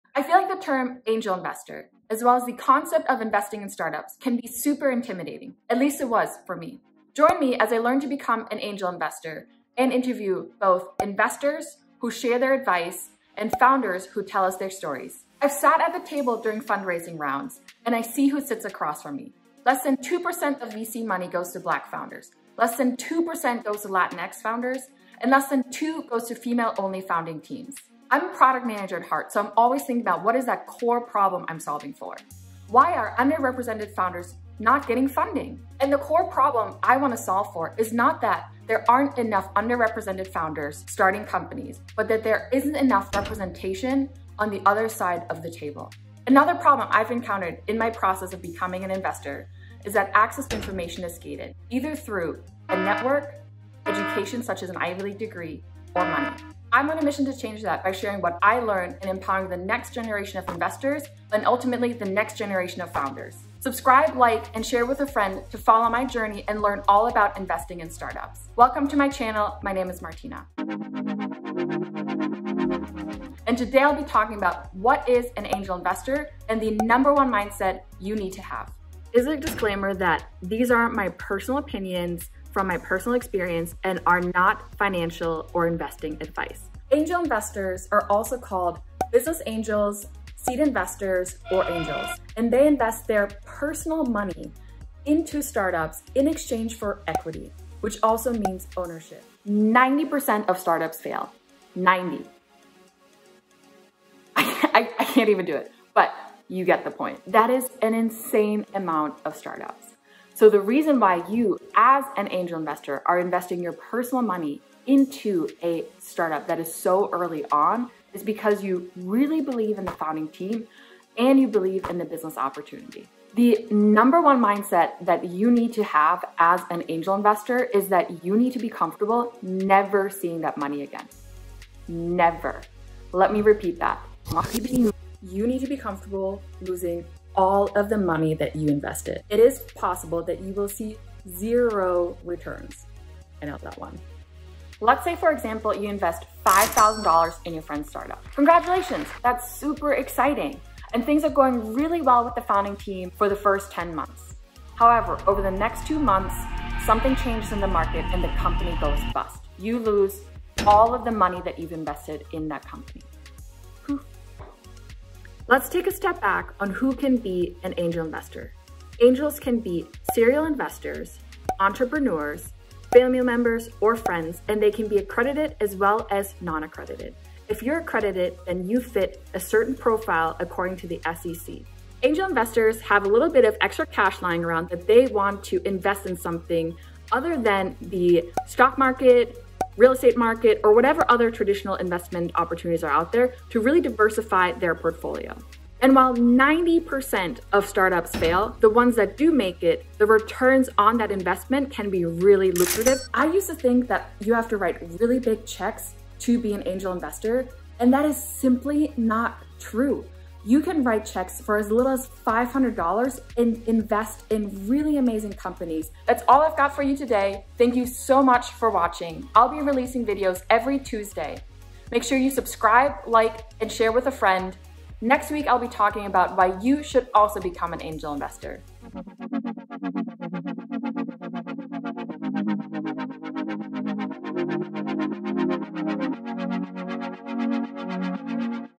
What is an ANGEL INVESTOR? #1 mindset you need for investing in start-ups
Angel investor explained! And the critical mindset an angel has to have when it comes to investing in early stage start-ups.

Hey everyone! It seems like everyone and their mom is or wants to be an angel investor but what does that really mean? This video is for you if
• you’ve always been curious and perhaps too afraid to ask
• just not sure who to ask
• you need a little refresher
Today, I’ll be breaking down the term angel investor, talk about why it can be so risky to invest in startups and the mindset that will get you far.

👋 SAY HI!